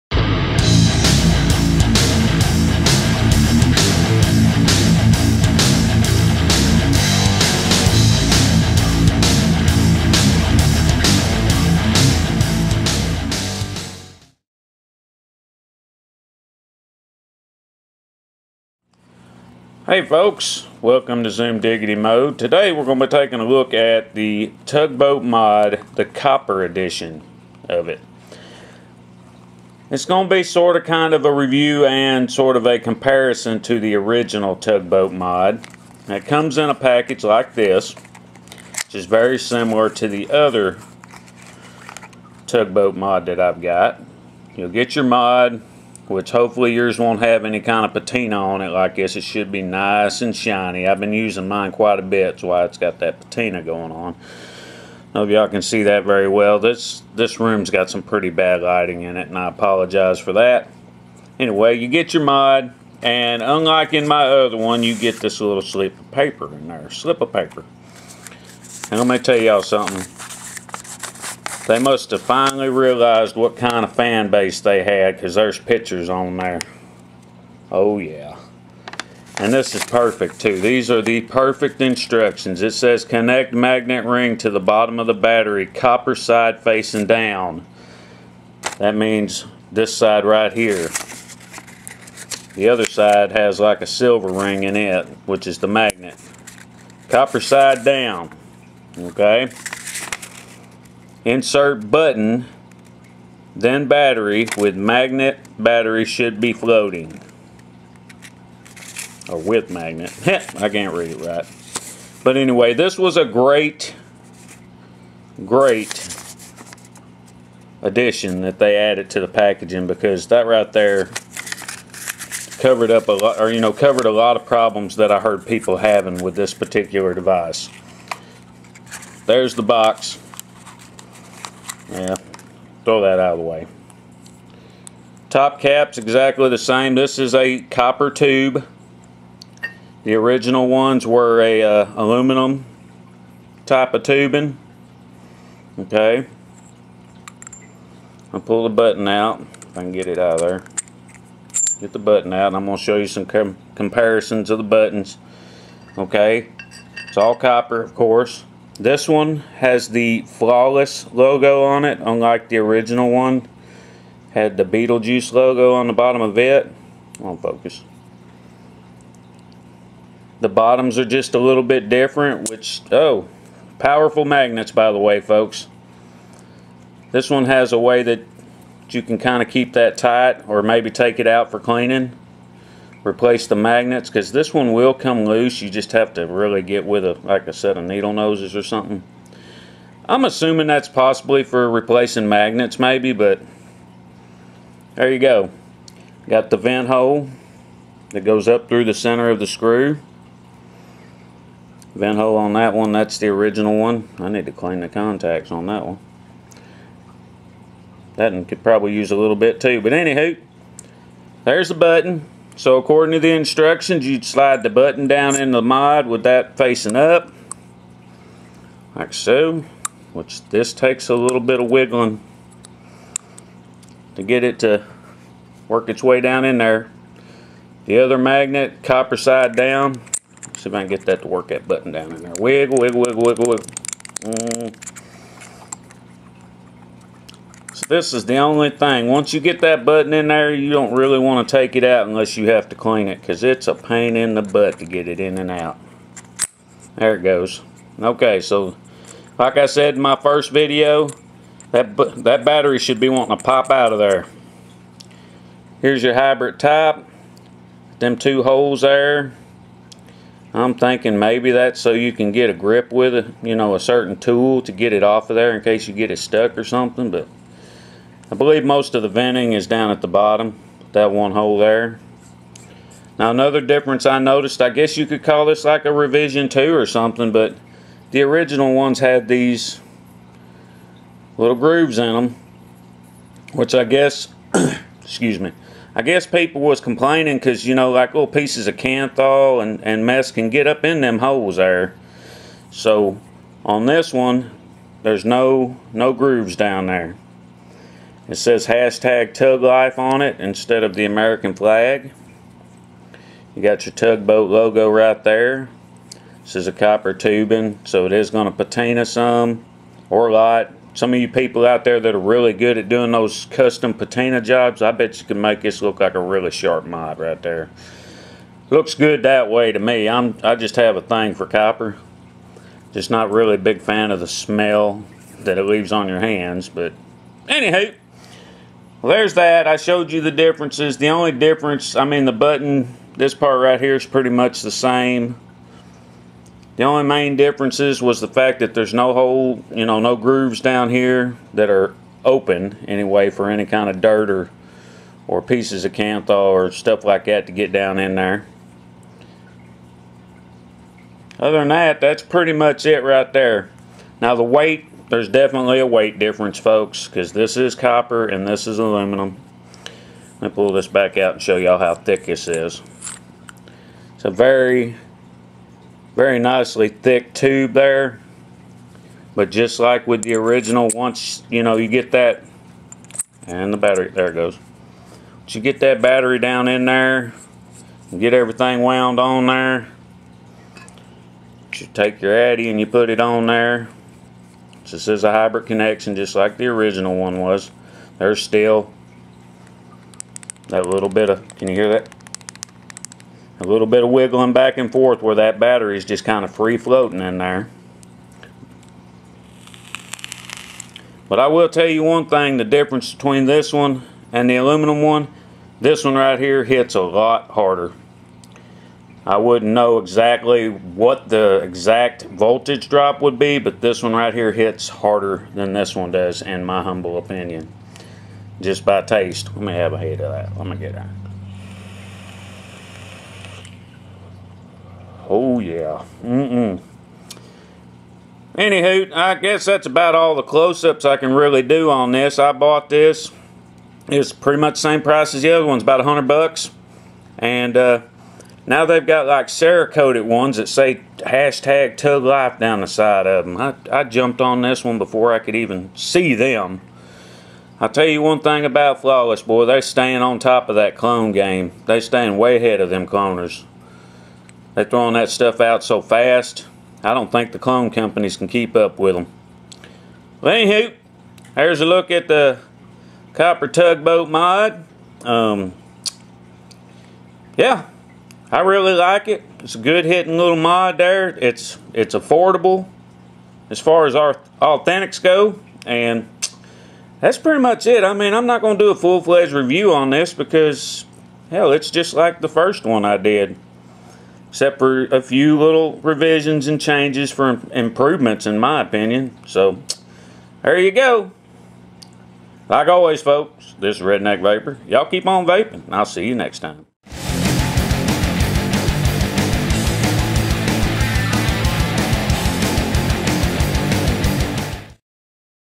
Copper Tugboat Mod by Flawless | TUGLYFE | Quick Look & Comparison to Original
In this video I'm giving you a quick look at my Copper Tugboat Mechanical Mod. The Tugboat Mod is a Collaboration between Beetle Juice and Flawless. If you would like to pick one up for yourself, they can be found
Machine out of Copper
22mm in diameter to fit most of your mod flush!
88.5mm in height
Uses 18650 battery
Hybrid connection for maximum conductivity
Copper contacts
Delrin Insulators
Flush magnetic button to give you a comfortable feel
Beautifully engraved with Tugboat Logo
Available now in Copper!